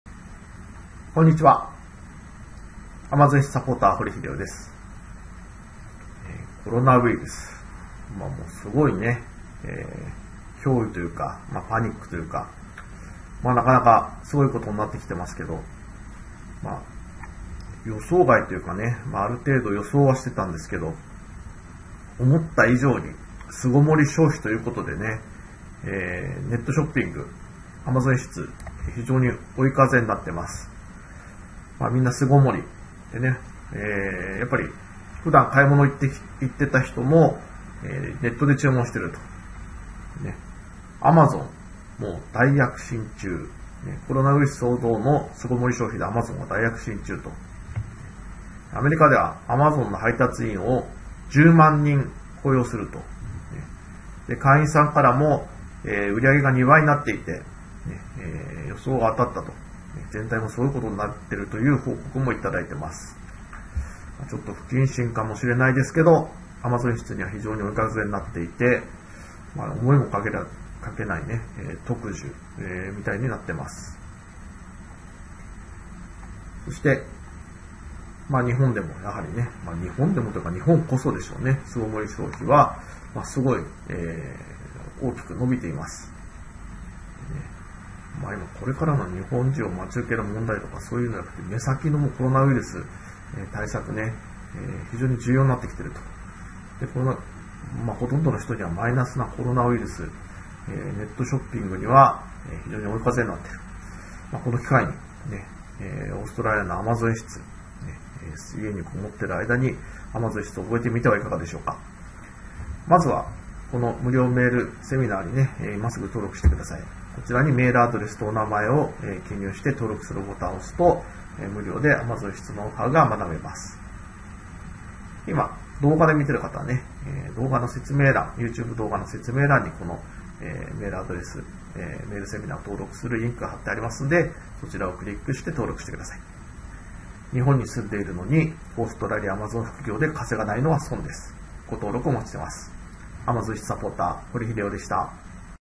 コロナウィルス騒動の巣篭もり消費でAmaozonが大躍進中
新型コロナウイルスの感染拡大で「巣ごもり消費」が増えている。米アマゾンは、新型コロナウイルス感染拡大に伴うオンラインショッピング需要の急増に対応し、10万人を新たに採用

1日10分で月10万円を稼ぐ! Amazon輸出ビジネスがわかる本(2013/11/22)